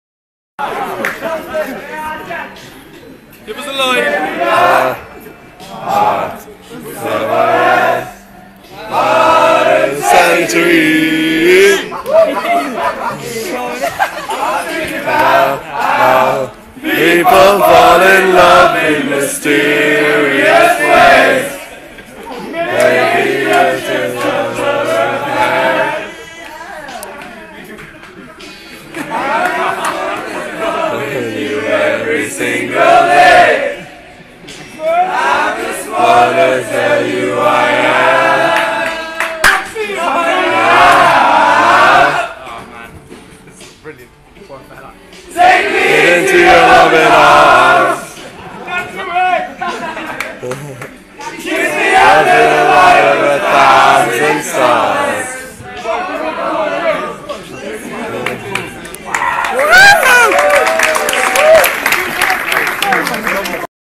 Schools Rugby Fans Sing To Ed Sheeran Lookalike Steward
From Munster Haka Schools